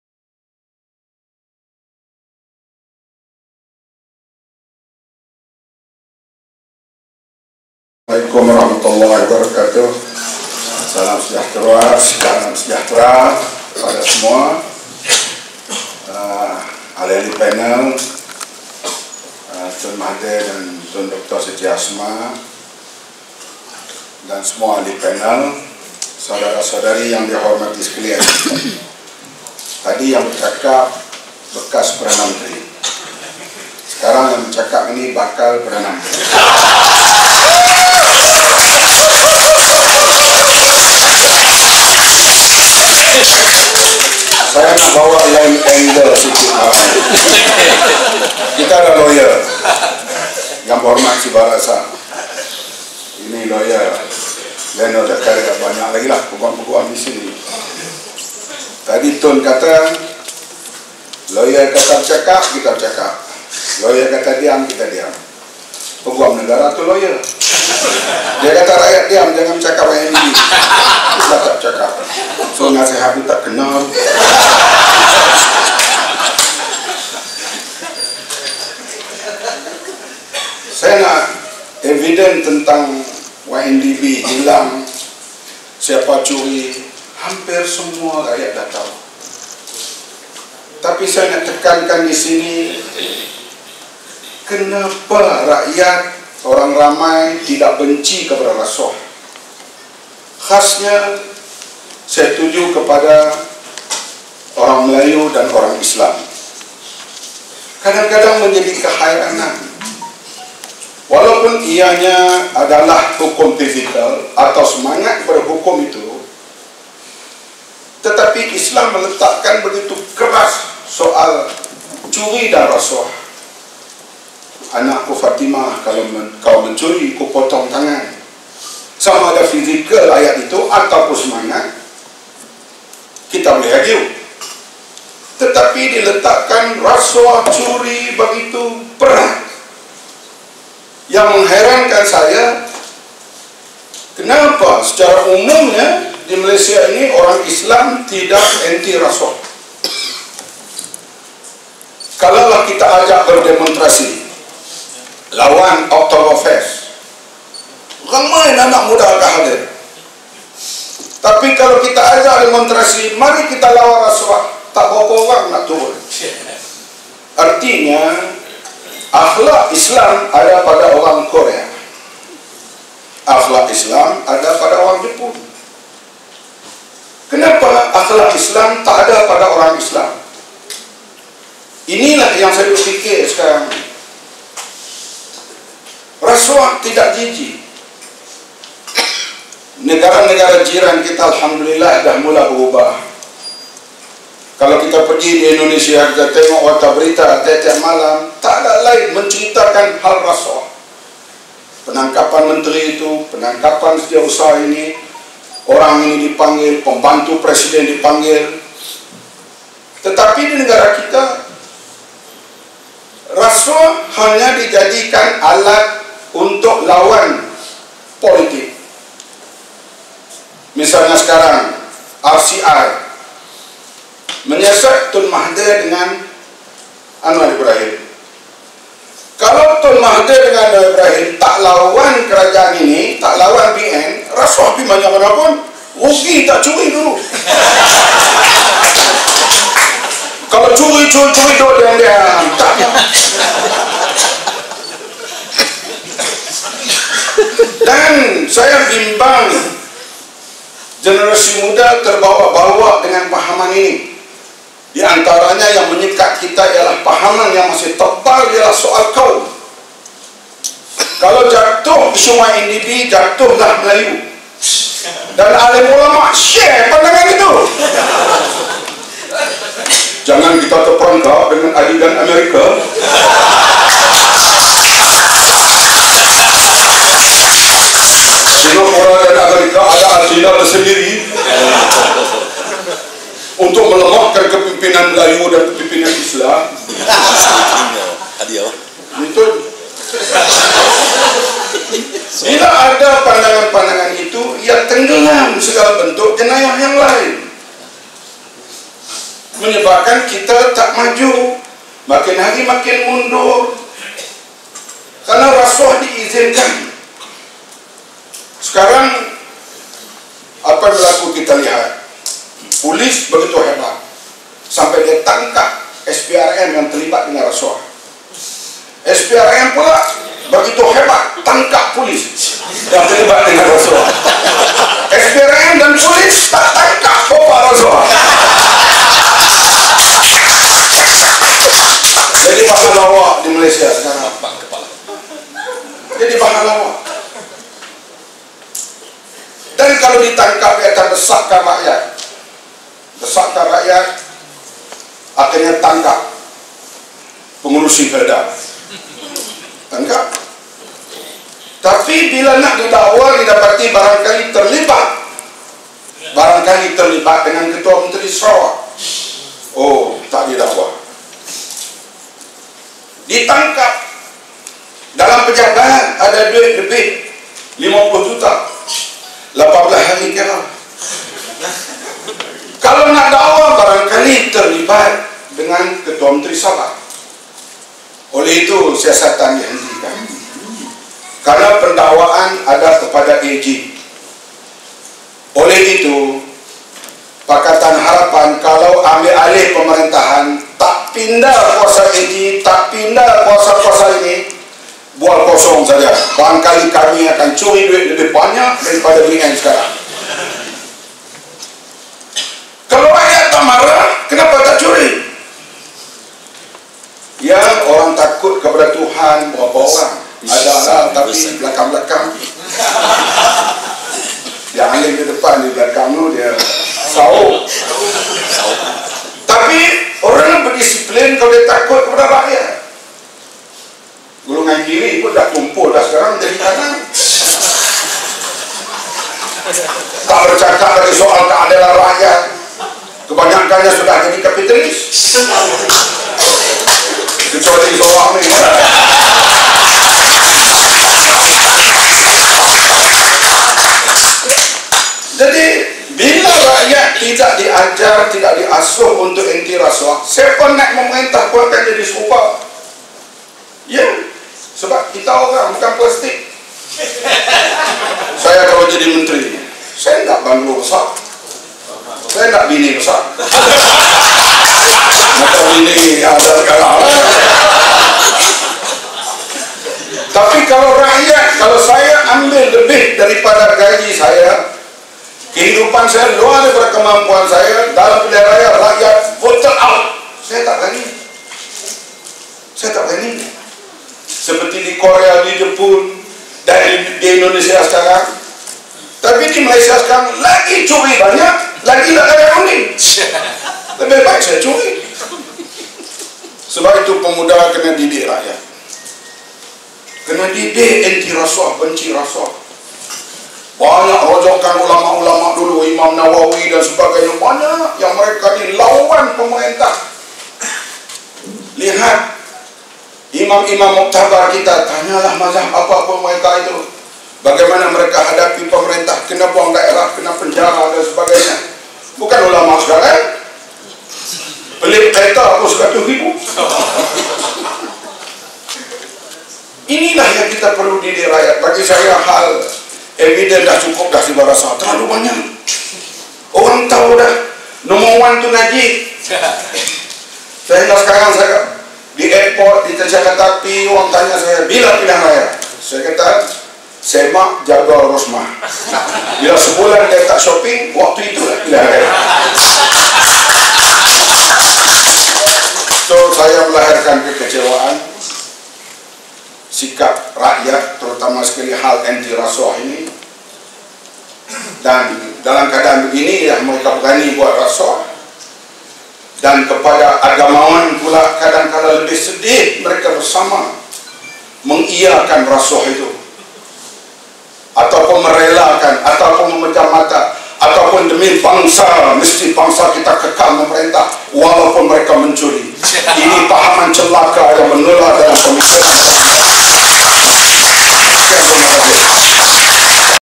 20171206 Kenapa orang Melayu Islam tidak anti rasuah
Kenapa orang Islam Melayu di Malaysia ini bersikap dingin terhadap anti rasuah? Kalau dianjurkan himpunan anti rasuah sudah pasti kehadiran nya kurang sekali. Dimana kah akidah dan akhlak Islam ? Adakah ini manjadi penyebab mengapa pemimpin yang rasuah dapat hidup selesa di Malaysia ?